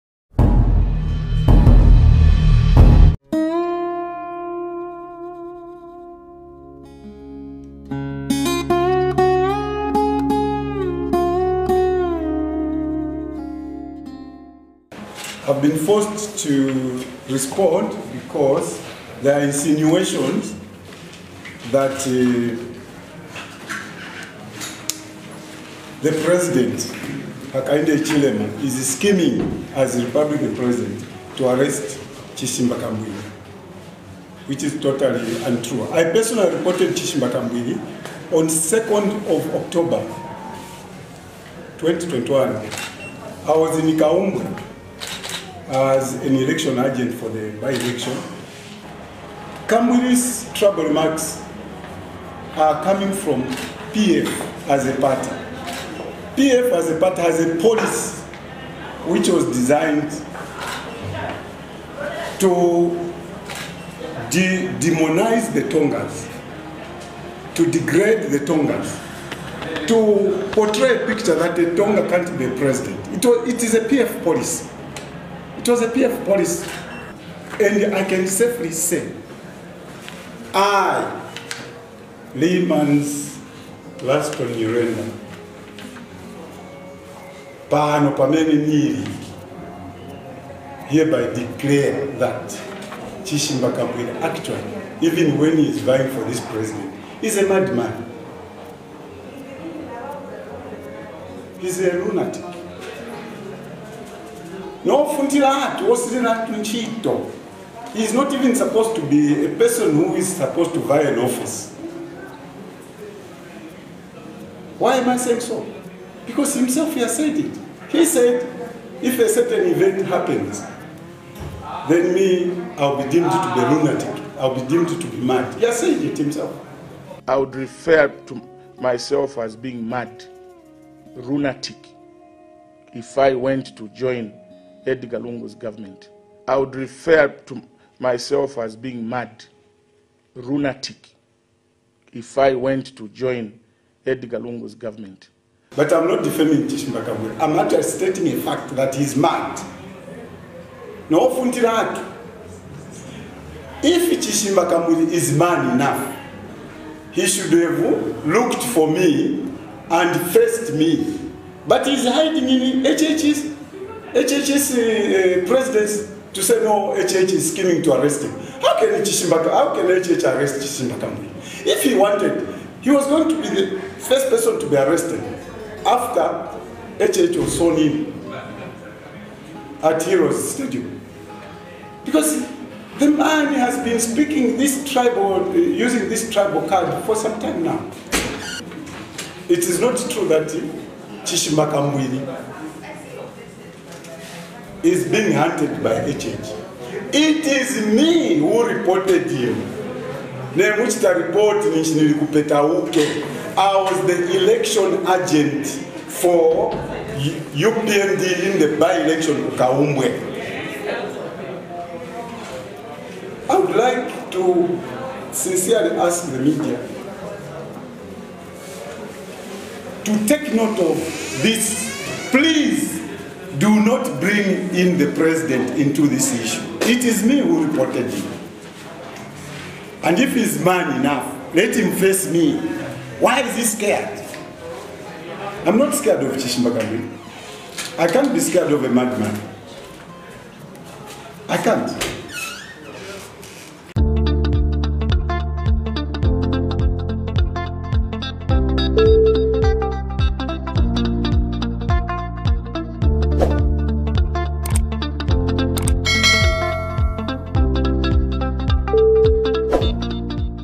KAMBWILI under fire for Preaching Tribalism - "If he's Man enough, let him face me" ~ LEEMANS
CONCERNED Zambian citizen and UPND member, Leenman's Nyirenda has asked Chishimba Kambwili to stop breeding falsehoods against President Hakainde Hichilema and face him instead.

Mr Nyirenda is the person who reported Mr Kambwili to the Police in Petauke falling his hate speech and tribal remarks in Kaumbwe.